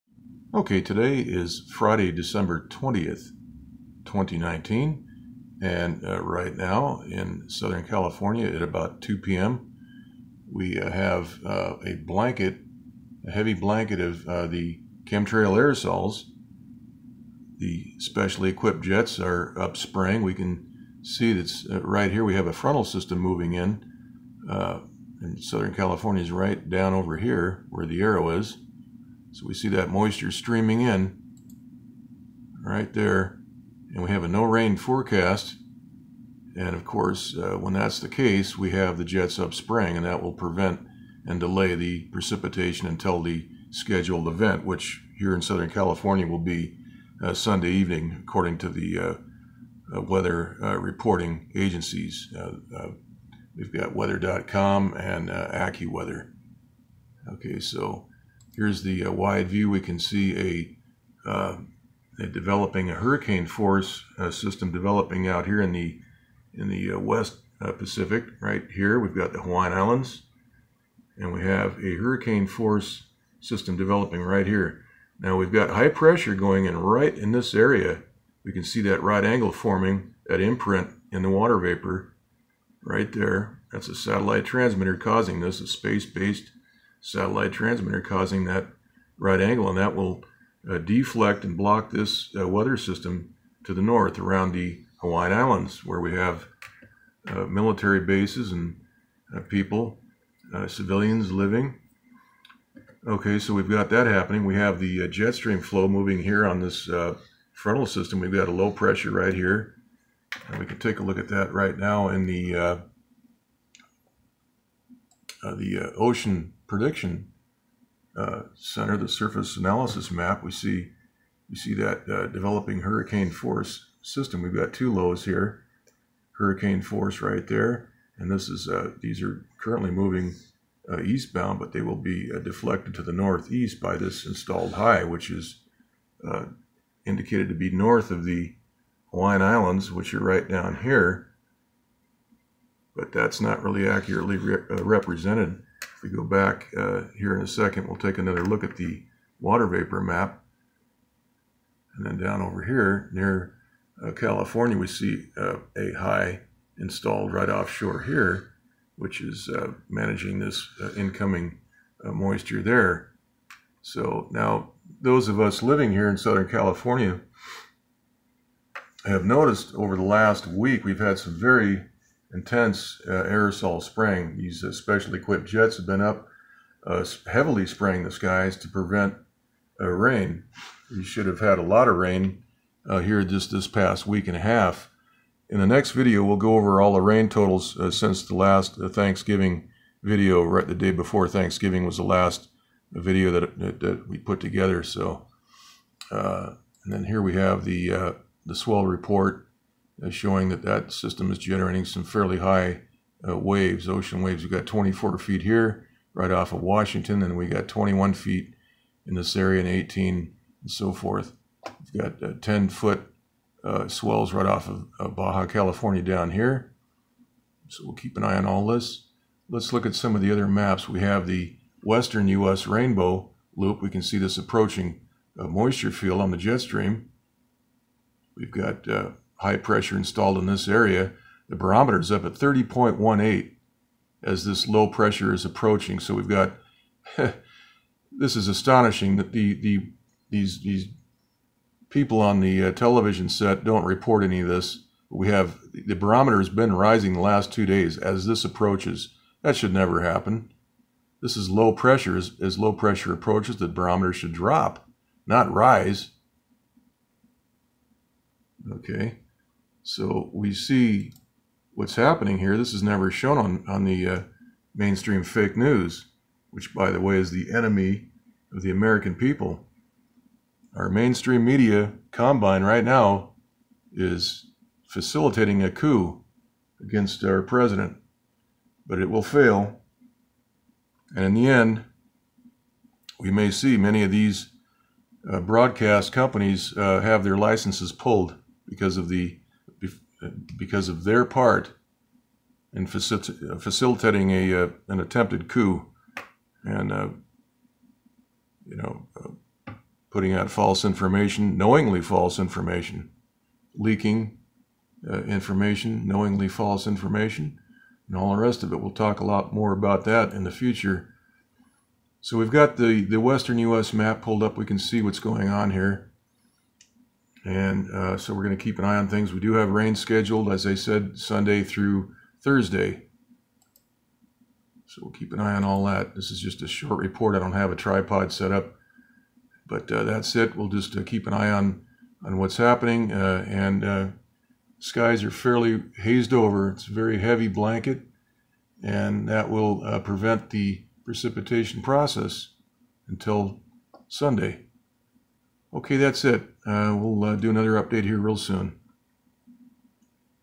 12-20-2019; Barometer Rising As Low Pressure Approaches! Heavy Aerosols Prevent Precipitation In CA
(Fri.) Heavy chemtrail aerosol spraying has been going on in the skies over California the past several days which is preventing the precipitation process while lowering the Relative Humidity. The aerosols cap the atmosphere preventing lift in the atmosphere; this manipulation of our weather also raises the temperature in the atmosphere. The fake news networks CBSNBCABCFOXCNNMSNBC blame this temperature rise on Carbon Dioxide emissions which has no factual basis. Rain/Precipitation has been scheduled for Sunday through Thursday according to the latest schedules. The Watervapor map shows a right angle imprint near the Hawaiian islands where high pressure is being installed by a satellite based transmitter; this will re-steer a developing hurricane force weather system to the NE and away from Hawaii.

The watervapor streaming into California the past week should have brought us rain over several days but the specially equipped jets were up spraying the skies with the desiccant aerosols 'chemtrails' which prevented the precipitation process.